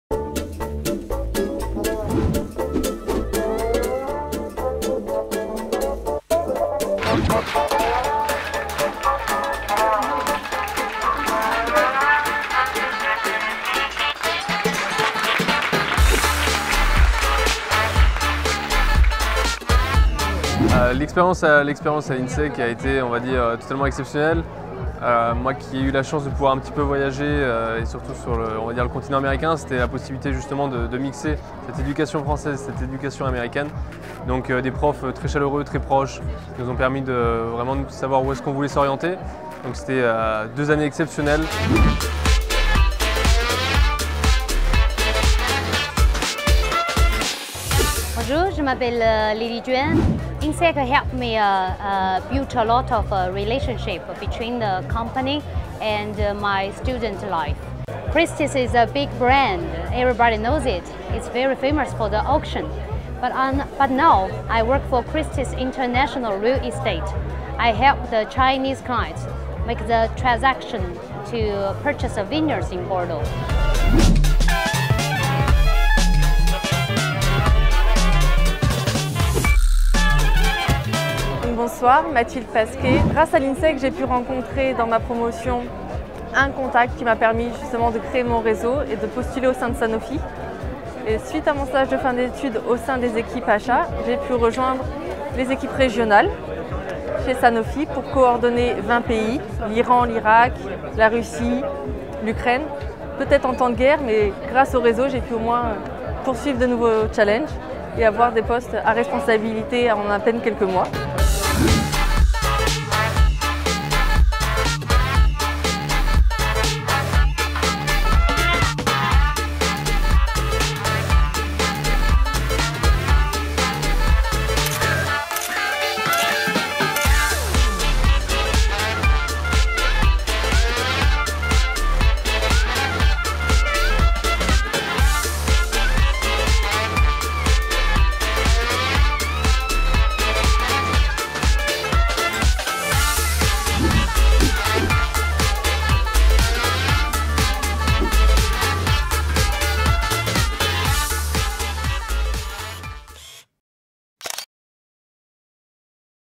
Remise de diplômes à l'INSEEC MSc & MBA Bordeaux - Promo 2013
Le 14 juin 2014, les diplômés de la Promotion 2013 de l'INSEEC MSc & MBA Bordeaux se sont retrouvés au sein de l'école pour se voir remettre leur diplôme de Master of Science ou de MBA.
Un évènement festif qui marque officiellement la fin de leurs études et leur entrée dans la vie active.